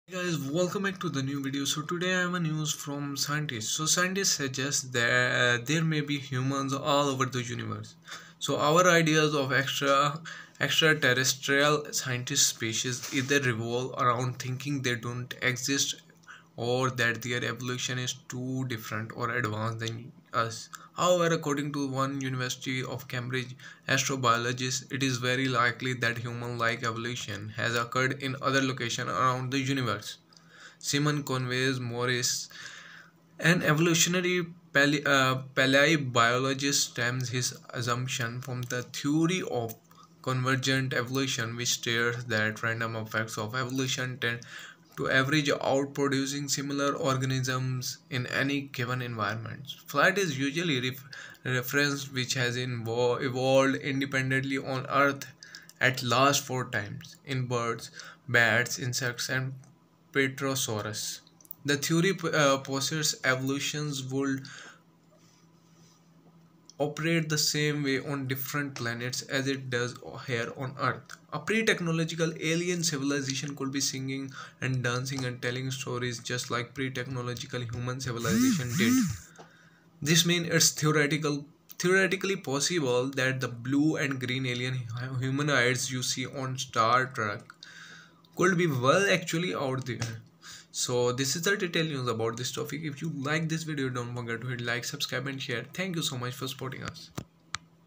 Scientist suggests there may be humans all over the universe | Search it more
Summary
🌌 Our ideas of extraterrestrial sentient species either revolve around thinking they don't exist or that their evolution is too different or advanced than us. However, according to one University of Cambridge astrobiologist, it is very likely that 'human like evolution' has occurred in other locations around the universe.

🧑‍🔬 Simon Conway Morris, an evolutionary palaeobiologist, stems his assumptions from the theory of convergent evolution, which states that random effects of evolution tend to average out producing similar organisms in any given environment. Flight is usually referenced, which has evolved independently on Earth at least four times - in birds, bats, insects and pterosaurs.

📰 The theory posits that evolution would operate the same way on different planets as it does here on Earth.

👉 A pre-technological alien civilization could be singing and dancing and telling stories just like pre-technological human civilization did. This means it’s theoretically possible that the blue and green alien humanoids you see on “Star Trek” could be, well, actually out there.

Follow our page for more daily content!